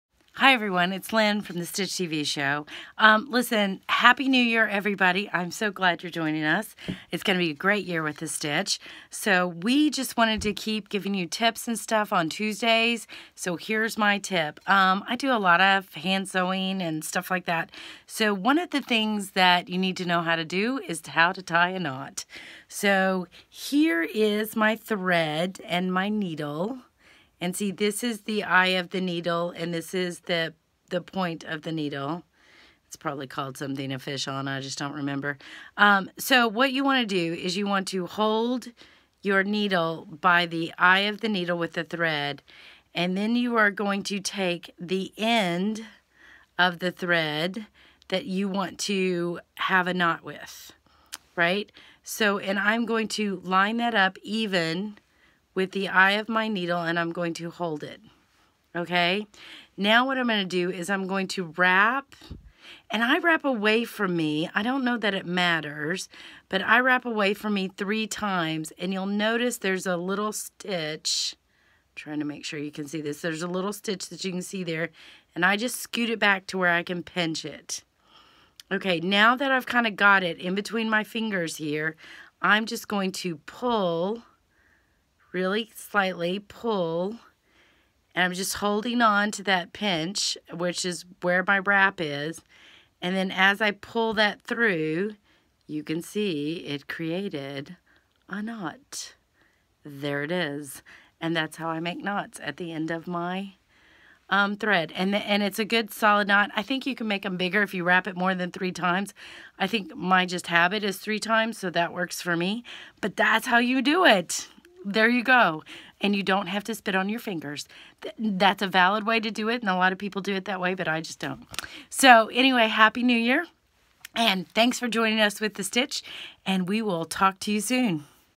Easy Way to Tie a Knot (for Sewing or Quilting)
Lynn's favorite way to tie a knot for hand stitching.